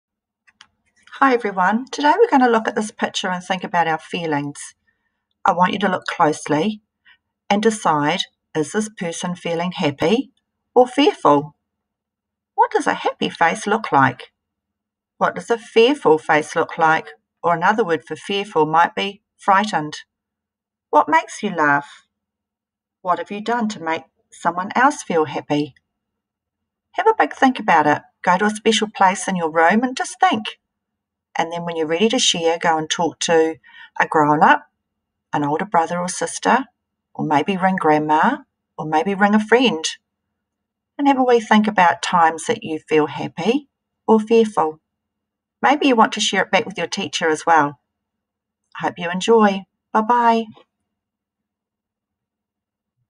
Happy or Fearful Writing Prompt with Mrs Lyon
How are you feeling today? What makes you feel happy or fearful?